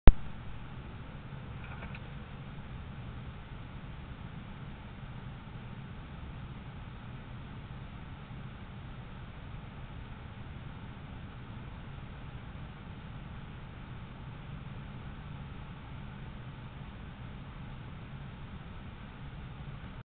Scaredy Cat Coyote
Wile E Coyote trying to get an apple in my live trap.